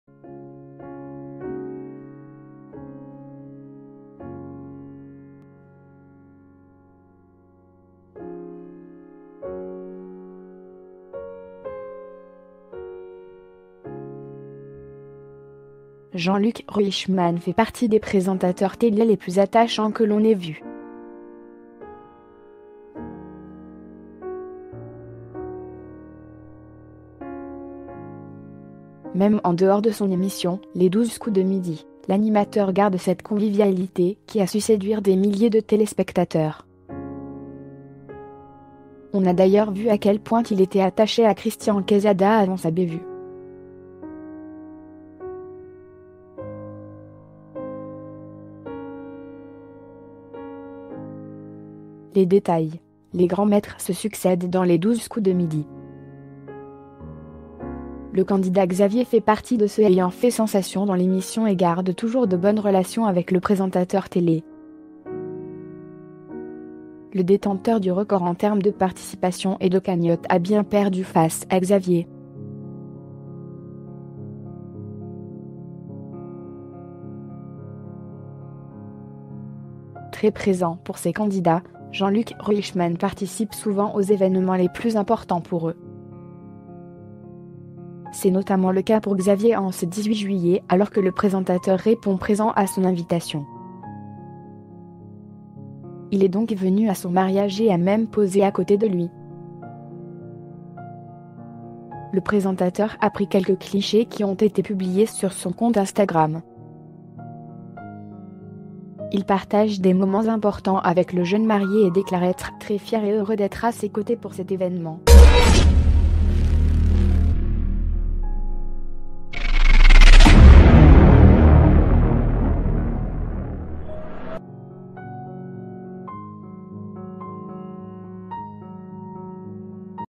Photo inédite : Jean Luc Reichmann présent au mariage de Xavier
,#a toi,#comedie,#funk you,#mister you (musical artist),#prison
Photo inédite : Jean Luc Reichmann présent au mariage de Xavier Jean Luc Reichmann fait partie des présentateurs télé les plus attachants que l’on ait vu. Même en dehors de son émission « Les 12 co